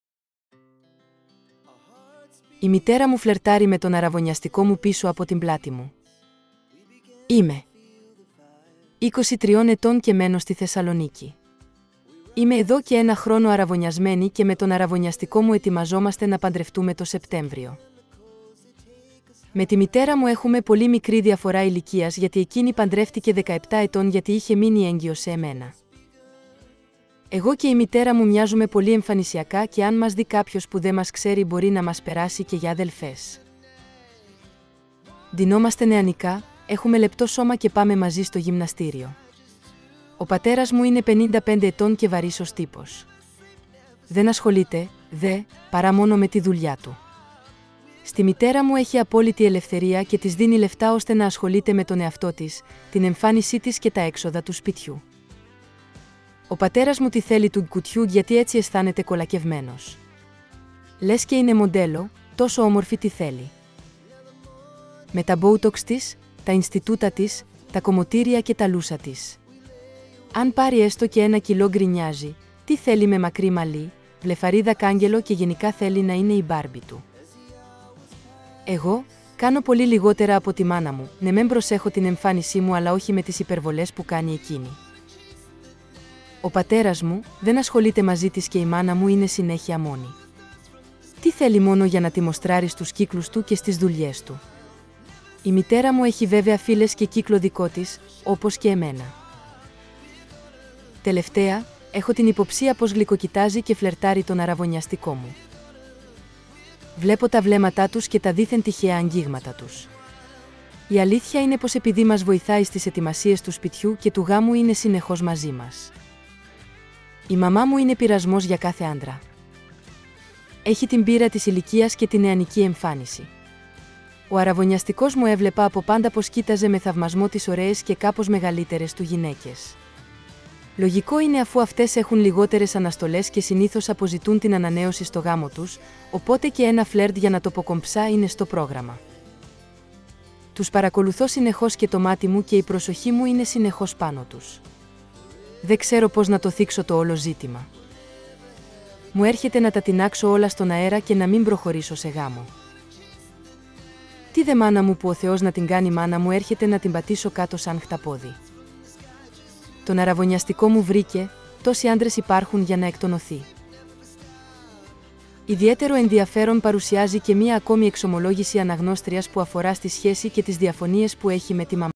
Η μητέρα μου φλερτάρει με τον αρραβωνιαστικό μου πίσω από την πλάτη μου!! √
23 ετών και μένω στη Θεσσαλονίκη. Είμαι εδώ και ένα χρόνο αρραβωνιασμένη και με τον αρραβωνιαστικό μου ετοιμαζόμαστε να παντρευτούμε το Σεπτέμβριο.
Με τη μητέρα μου έχουμε πολύ μικρή διαφορά ηλικίας γιατί εκείνη παντρεύτηκε 17 ετών γιατί είχε μείνει έγκυος σε εμένα.

Εγώ και η μητέρα μου μοιάζουμε πολύ εμφανισιακά και αν μας δει κάποιος που δε μας ξέρει μπορεί να μας περάσει και για αδελφές.

Ντυνόμαστε νεανικά, έχουμε λεπτό σώμα και πάμε μαζί στο γυμναστήριο. Ο πατέρας μου είναι 55 ετών και βαρύς ως τύπος. Δεν ασχολείται, δε, παρά μόνο με τη δουλειά του. Στη μητέρα μου έχει απόλυτη ελευθερία και της δίνει λεφτά ώστε να ασχολείται με τον εαυτό της, την εμφάνισή της και τα έξοδα του σπιτιού.